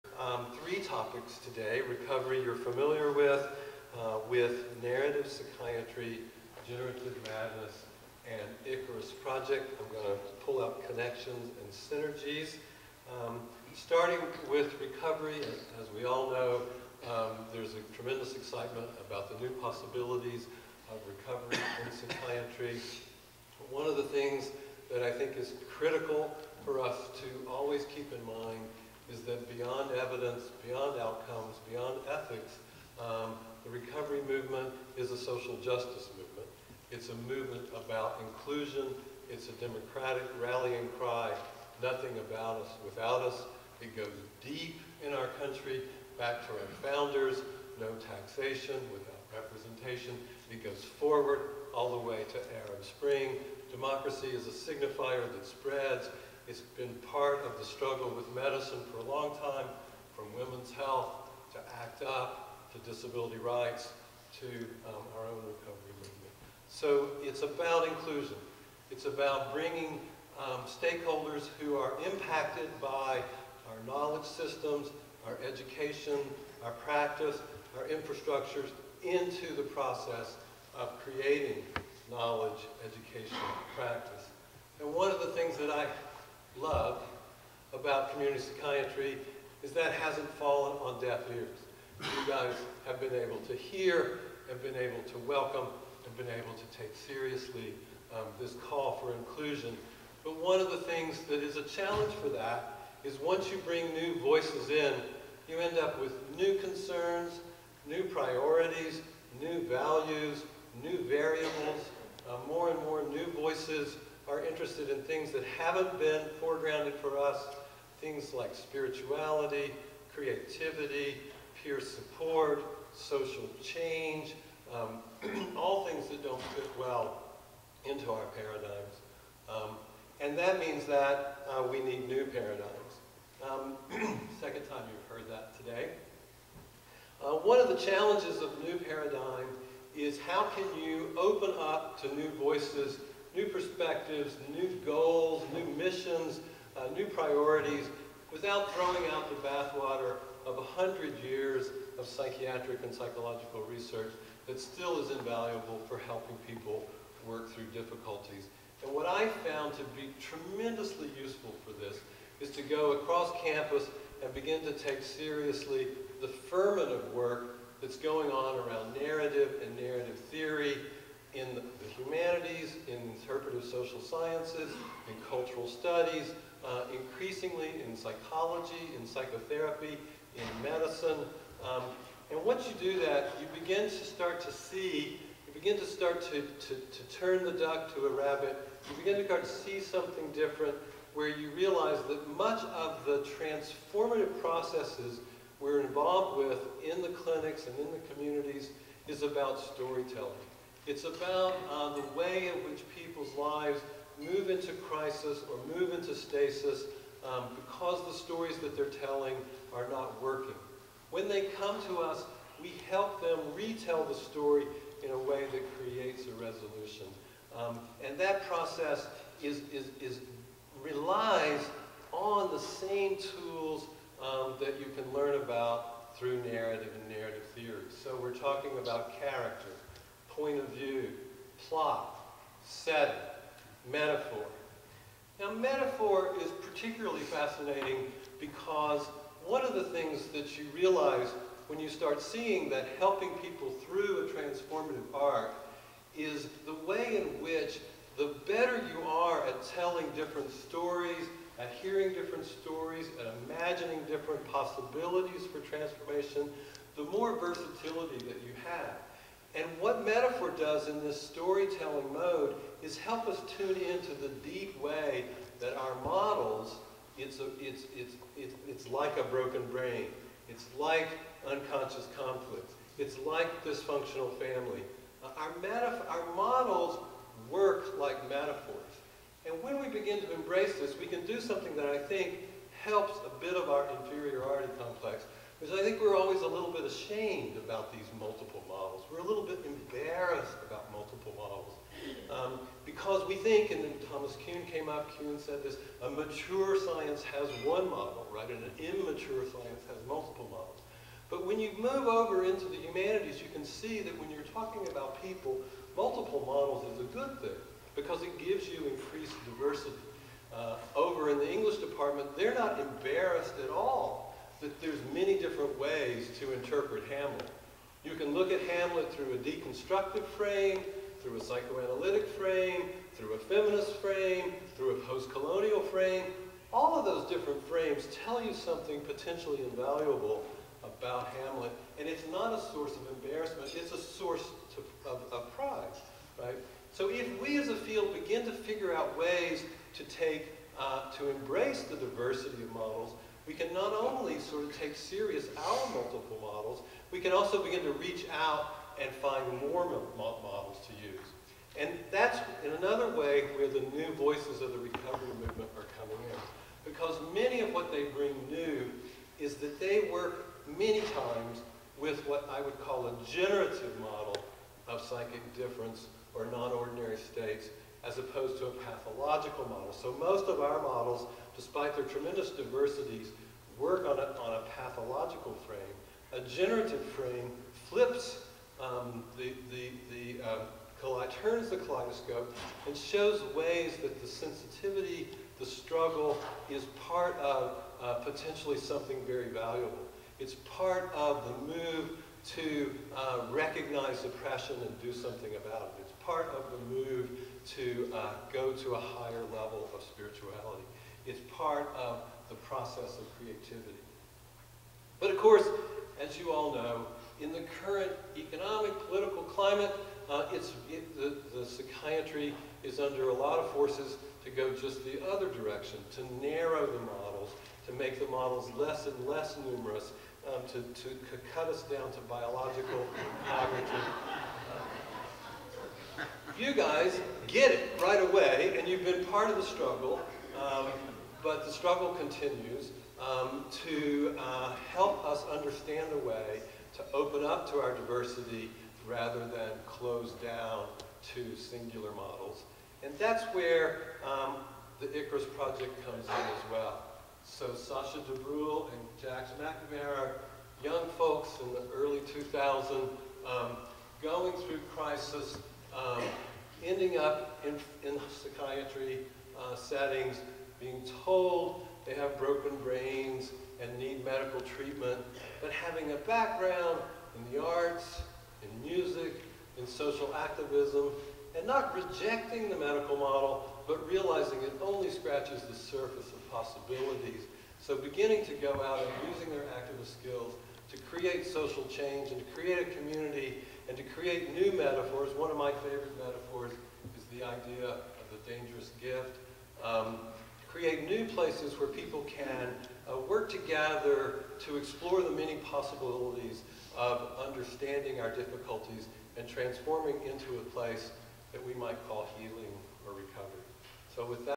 Brad Lewis at the AACP
Professor Bradley Lewis speaking about narrative psychiatry at the American Association of Community Psychiatrists